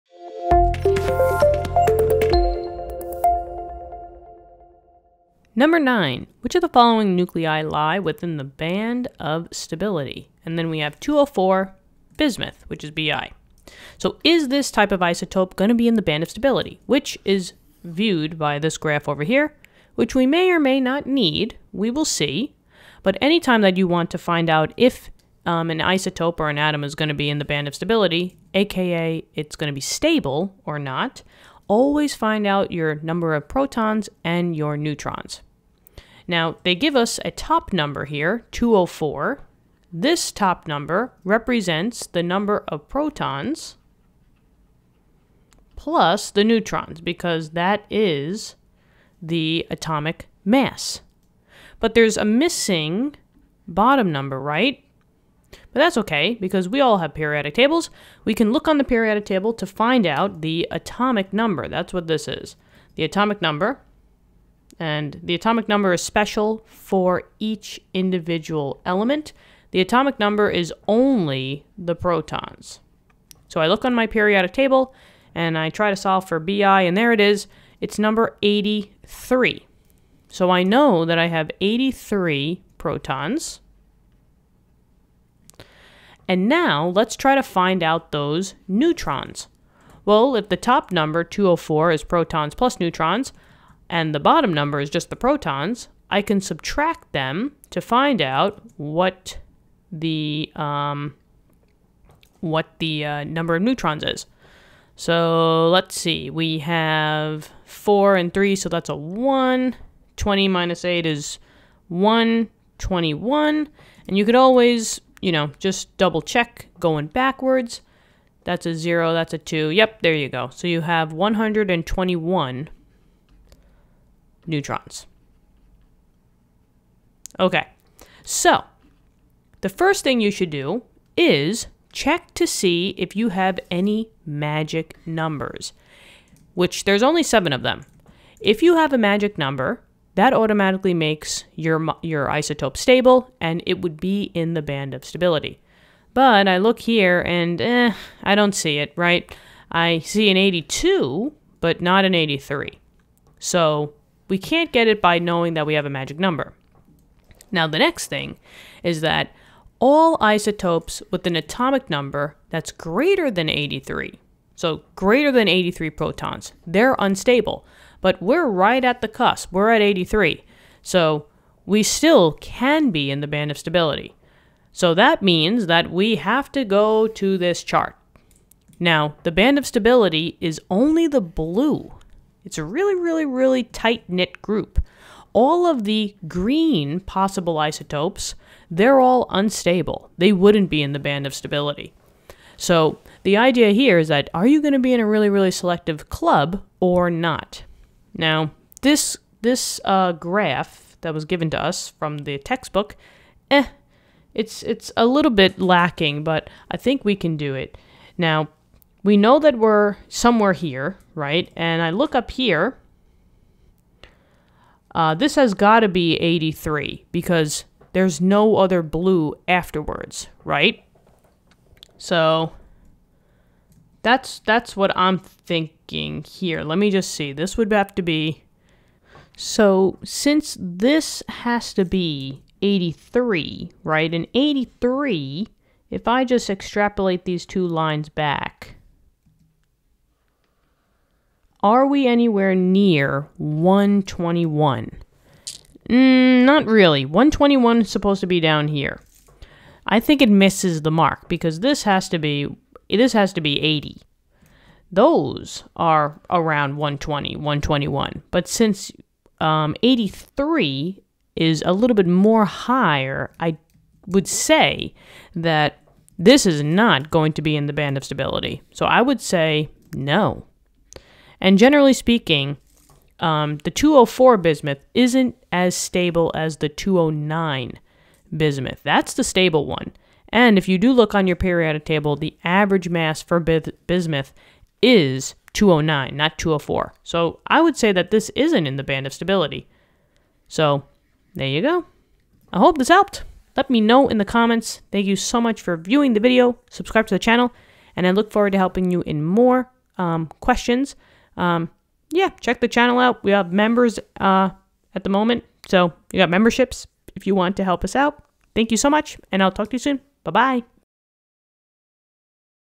21.9c | How to determine if Bi-204 is a stable isotope
Which of the following nuclei lie within the band of stability shown in Figure 21.2?
204 Bi